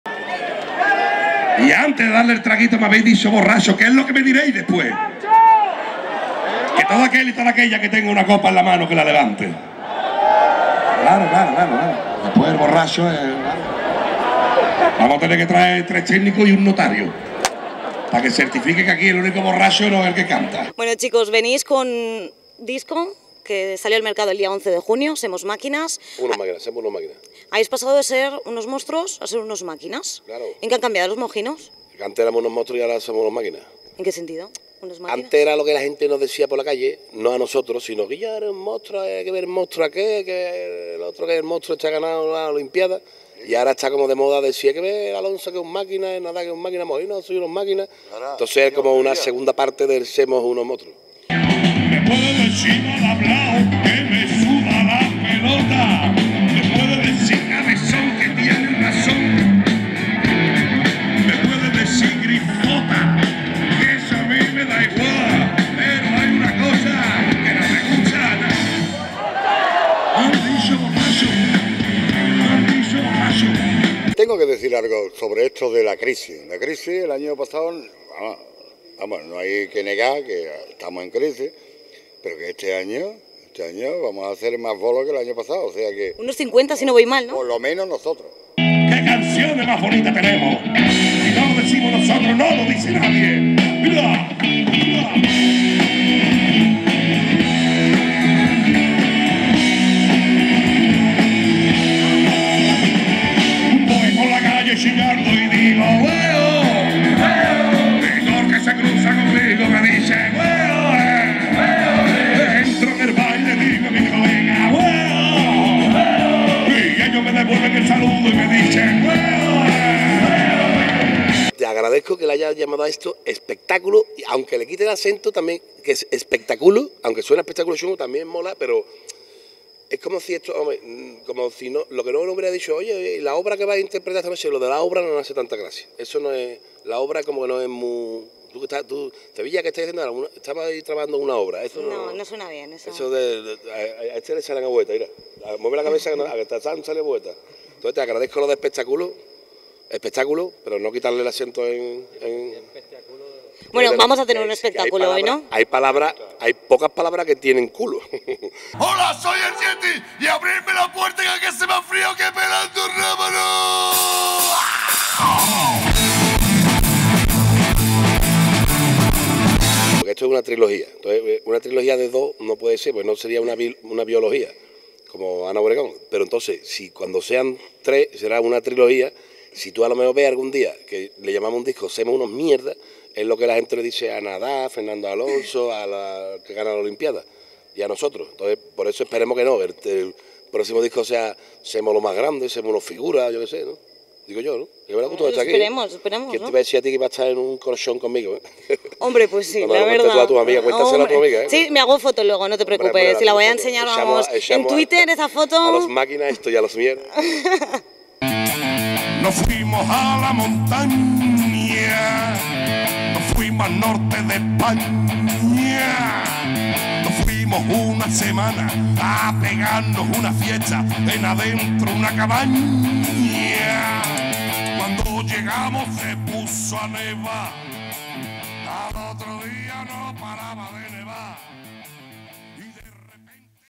La Comarca.tv - Mojinos Escozíos en Caspe
Uno de los conciertos estrella en las fiestas de Caspe ha sido el ofrecido por "Mojinos Escozíos" el miércoles 14 de agosto de 2013. Sandrá Buzón, periodista del grupo La COMARCA, estuvo con ellos momentos previos al espectáculo. Tercera vez que este grupo toca en la Ciudad del Compromiso y de nuevo el público vibró a ritmo de su mejor rock and roll.